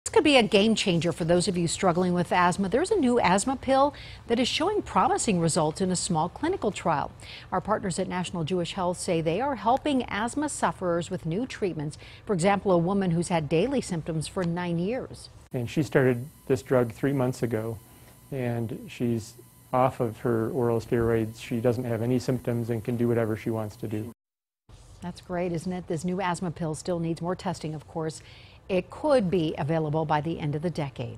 Asthma Control
Several new asthma treatments are showing promise.